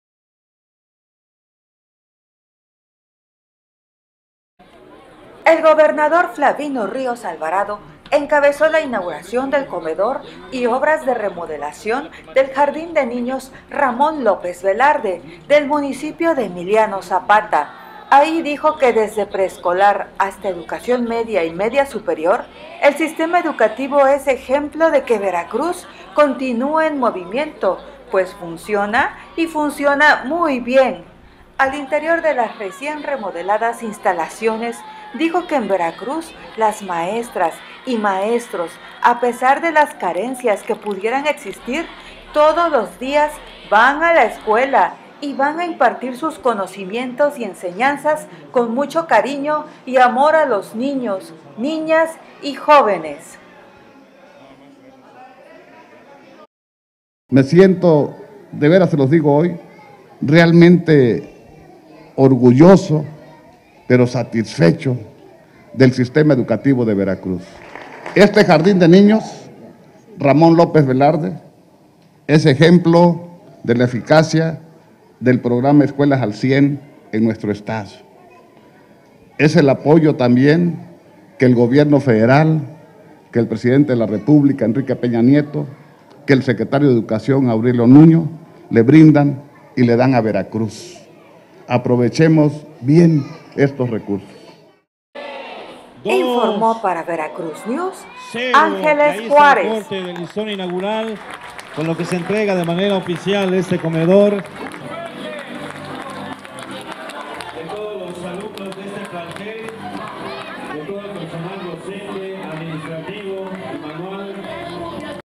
Veracruz está de pie y el sistema educativo es el mejor ejemplo: Flavino Ríos
Anuncia próxima dispersión de compensación pendiente a maestros del programa Escuelas al Cien

Ojo de Agua, Mpio. Emiliano Zapata, Ver., 10 de noviembre de 2016.- Desde preescolar hasta educación media y media-superior, el sistema educativo es ejemplo de que Veracruz continua en movimiento, pues funciona y funciona muy bien, manifestó el Gobernador Flavino Ríos Alvarado durante la inauguración del comedor y obras de remodelación del Jardín de Niños Ramón López Velarde.

Durante el acto protocolario, el mandatario aseveró que Veracruz está de pie y trabajando; “los veracruzanos hacemos nuestra vida con toda normalidad y con toda cotidianeidad”.

En este tenor, destacó que el sistema educativo es ejemplo de ello, de manera especial, el Jardín de Niños Ramón López Velarde, ejemplo de la eficacia del programa Escuelas al Cien en la entidad.

“Es el apoyo que el Presidente de la República, Enrique Peña Nieto, y el secretario de Educación Pública, Aurelio Nuño Mayer, le brindan a Veracruz; aprovechemos bien estos recursos, hagamos bien las cosas”.

Al interior de las recién remodeladas instalaciones, dijo que en Veracruz, las maestras y maestros, a pesar de las carencias que pudieran existir, “todos los días, de lunes a viernes, van a la escuela y van a impartir sus conocimientos y enseñanzas con mucho cariño y amor a nuestros niños, niñas y jóvenes”.

Anunció que en el caso del pendiente que se tiene con la compensación a docentes que participan dentro de Escuelas al Cien, “en una hora, hora y media, se debe estar dispersando esta compensación para que sigamos trabajando con más entusiasmo, con más ganas, y sigamos mejorando nuestras escuelas”.

Finalmente, llamó a los profesores a continuar con el buen desempeño mostrado hasta el momento, pues ellos son los encargados del 50 por ciento de la formación de los veracruzanos.

“De verdad se los digo, cuando uno es un ciudadano útil al estado o al país, siempre se lo reconoce uno primero a la mamá, al papá; luego a las maestras y los maestros. Muchas felicidades”.

A esta inauguración también asistieron: los secretarios de Gobierno y Educación estatal, Genaro Mejía de la Merced y Xóchitl Adela Osorio Martínez, respectivamente; los subsecretarios de Educación Media-Superior y Superior, Denisse Uscanga Méndez, y de Desarrollo Educativo, Nemesio Domínguez Domínguez.

Además, los directores generales de Educación Inicial y Preescolar, Rosalinda Irene García Mota; del Instituto de Espacios Educativos del Estado de Veracruz, Natalia Callejas Martínez, y del Jardín de Niños Ramón López Velarde, Blanca Elvia Silva Granados; así como los alumnos del tercer grado grupo B, Dafne Johana González Zúñiga y Antony Viveros Aguilar.